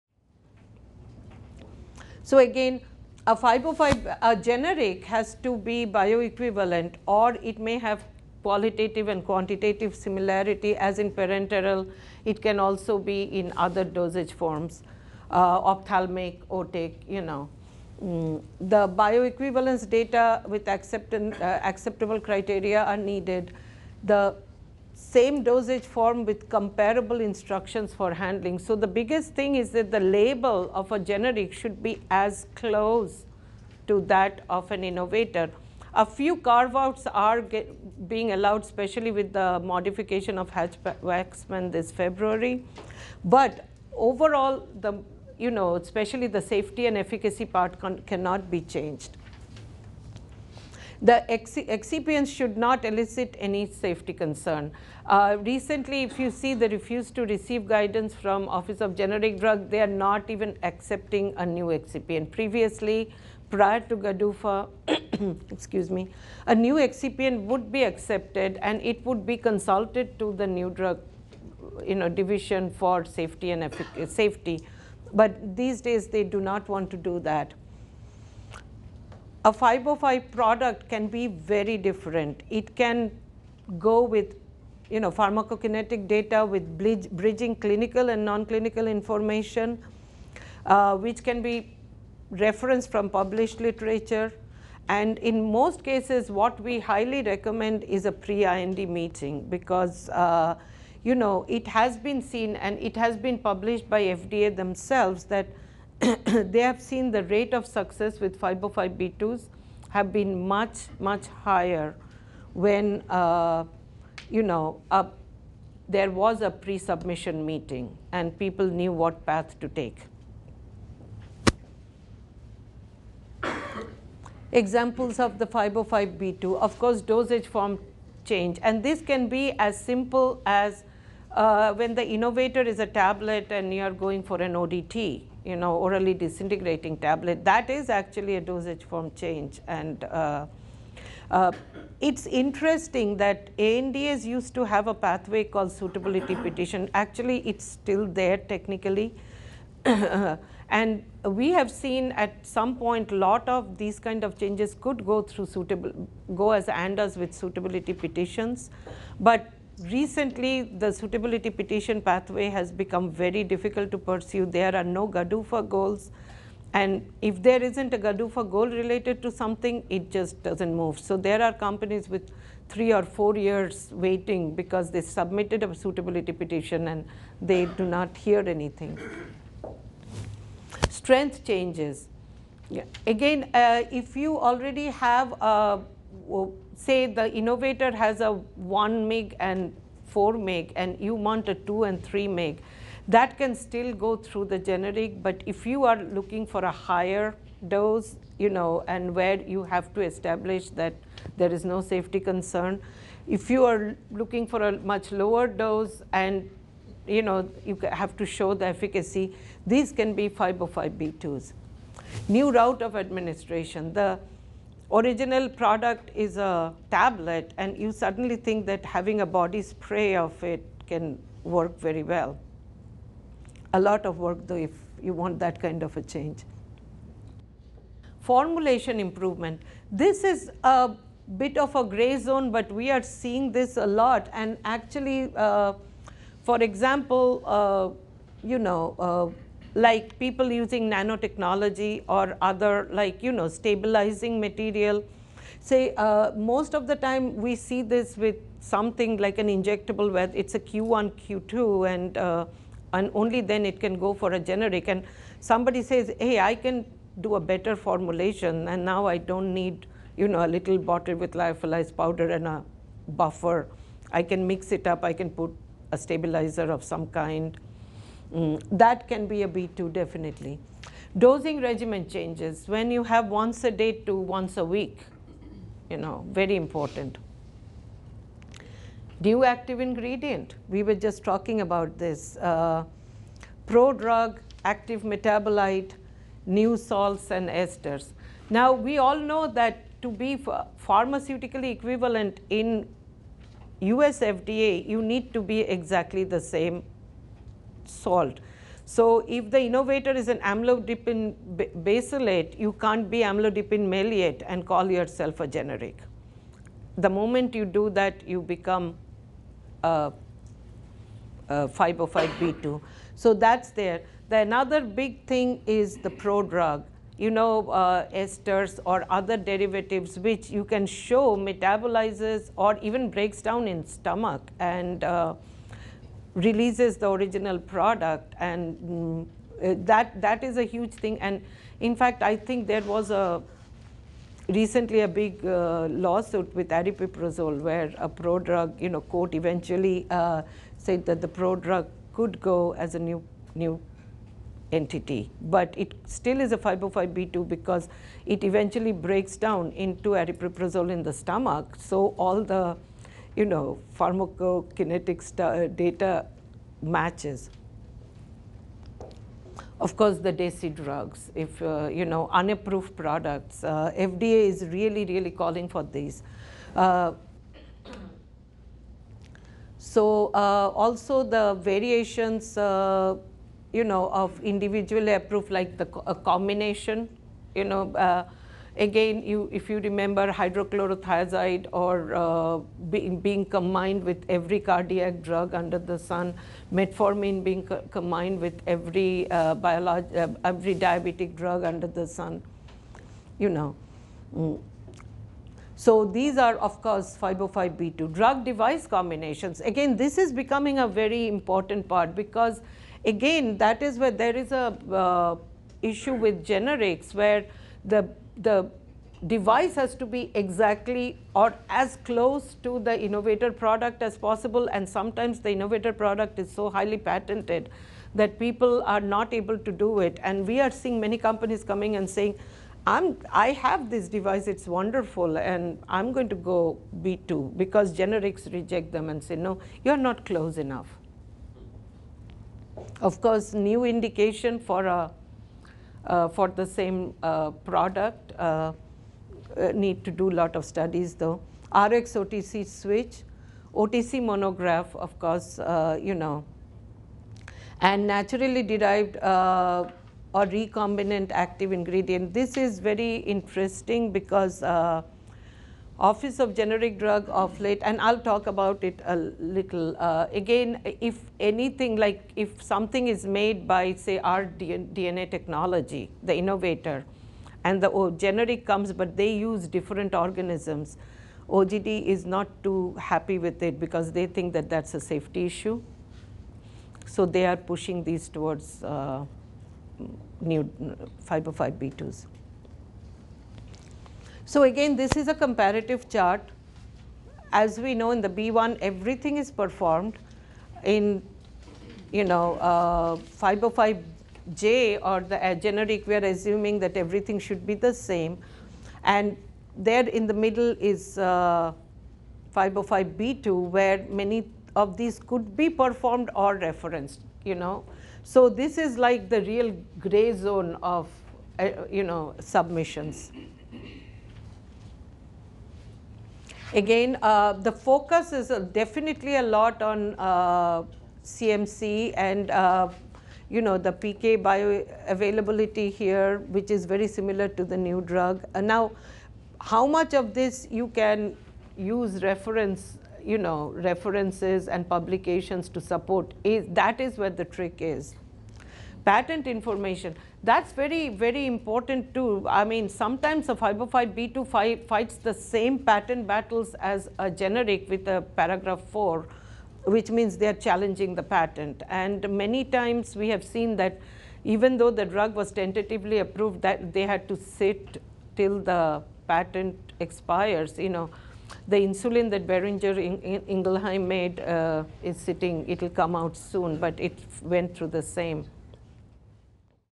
FDA 505(b)(2) Applications Seminar Part II: The Regulatory Pathway
Regis Technologies hosted a seminar on FDA 505(b)(2) Applications in March 2016. Two guest speakers from PAREXEL Consulting, Aloka Srinivasan, Ph.D., and Partha Roy, Ph.D., delivered excellent information and led an engaging discussion on this convenient New Drug Application (NDA) approval pathway for the pharmaceutical industry. Watch this segment for Dr. Srinivasan's examples of and differentiation between 505(b)(1), 505(b)(2), and 505(j) applications.